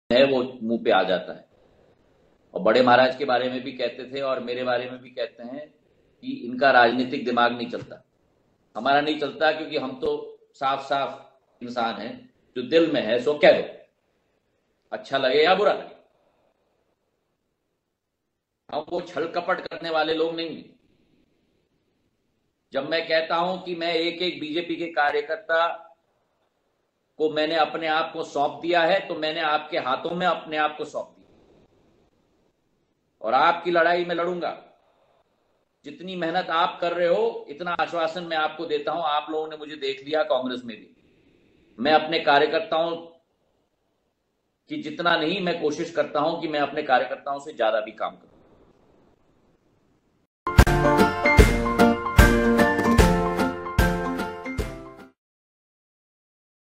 मेरे पूज्य पिताजी हो या मैं हमने कभी भी राजनीति में छल कपट का सहारा नहीं लिया: ज्योतिरादित्य सिंधिया
चाहे मेरे पूज्य पिताजी हो या मैं हमने कभी भी राजनीति में छल कपट का सहारा नहीं लिया, इसीलिए लोग हम पर अनर्गल आरोप लगाते है। मैंने खुद को पूरे विश्वास के साथ भारतीय जनता पार्टी को सौंप दिया है अब यही मेरा परिवार है।